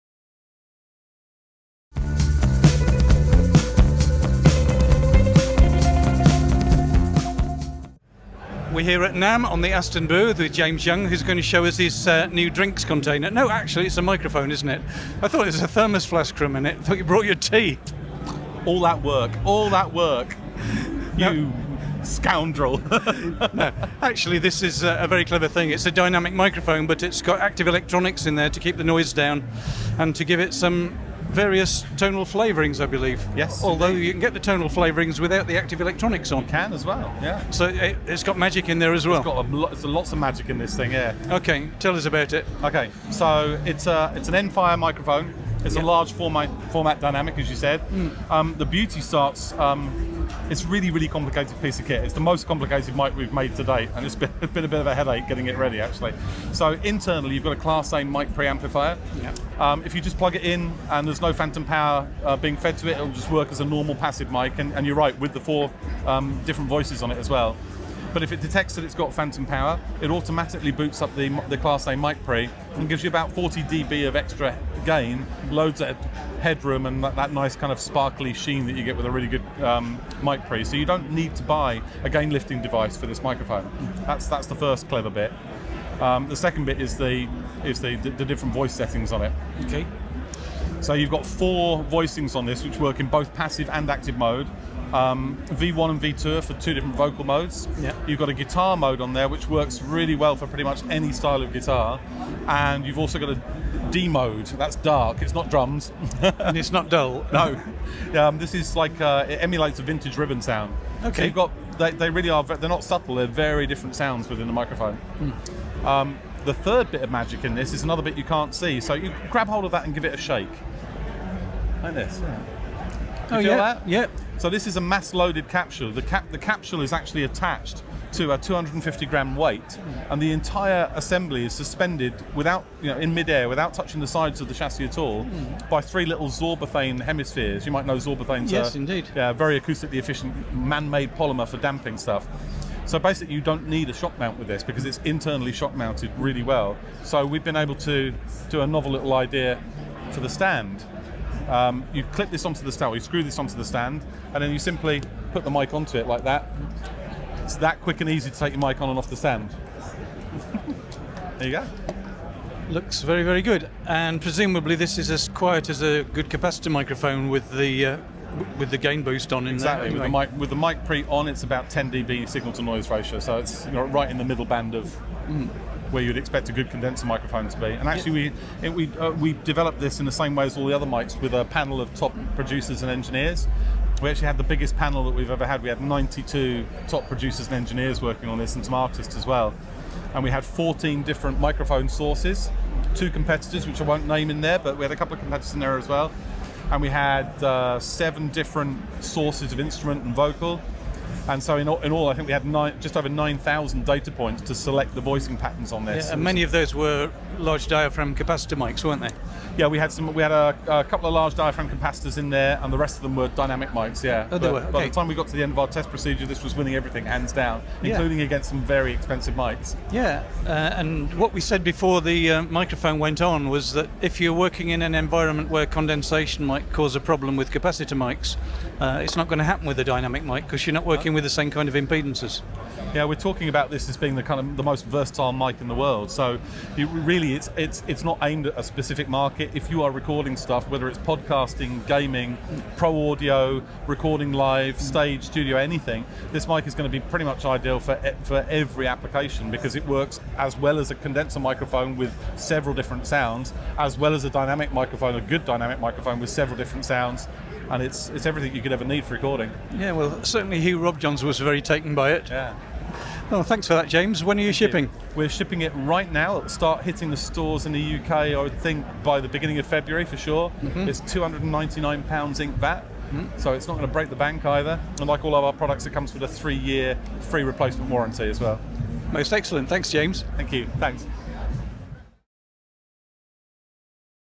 Aston Microphones Stealth - NAMM 2019
Large format dynamic microphone with 4 switchable voices.